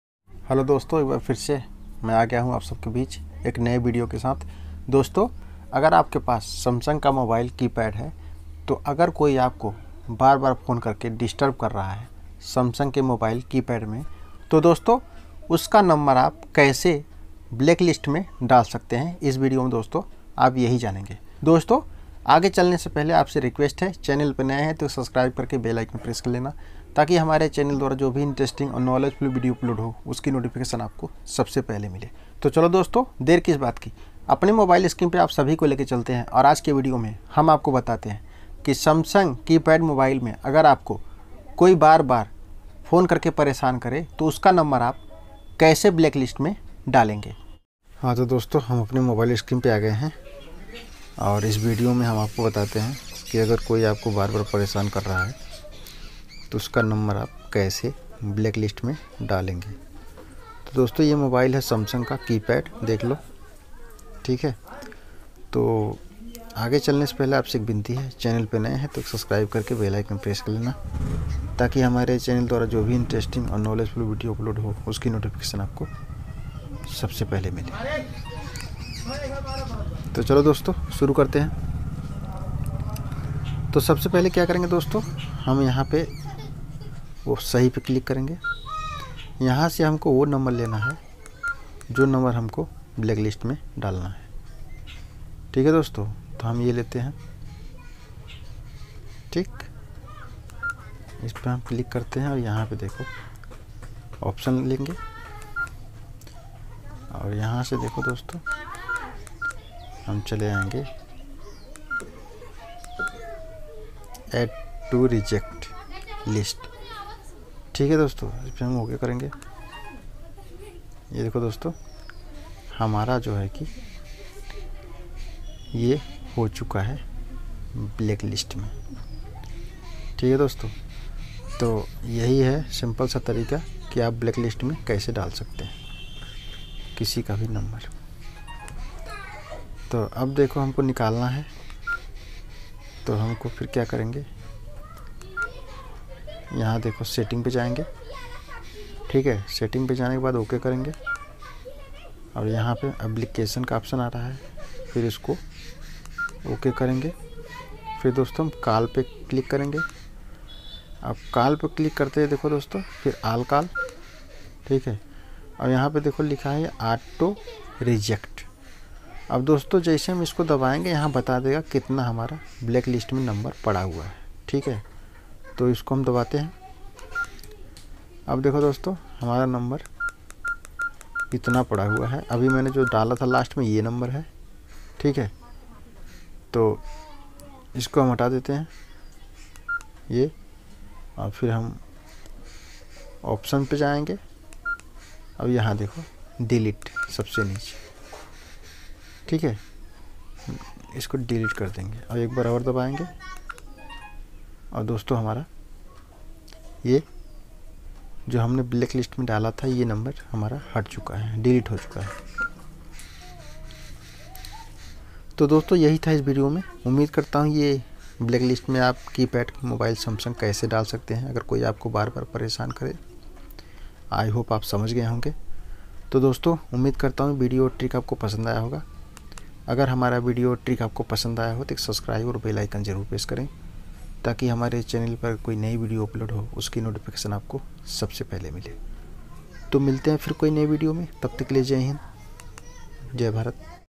Samsung मोबाइल में ब्लैक लिस्ट में नंबर कैसे डालें |how to put in blacklist in samsung keypad mobile
how to put number in blacklist in samsung keypad mobile

Samsung mobile keypad mein blacklist number kaise daale

hello dosto is video mein bataya gaya hai ki aap apne Samsung keypad mobile mein Blacklist mein number kaise dal sakte hain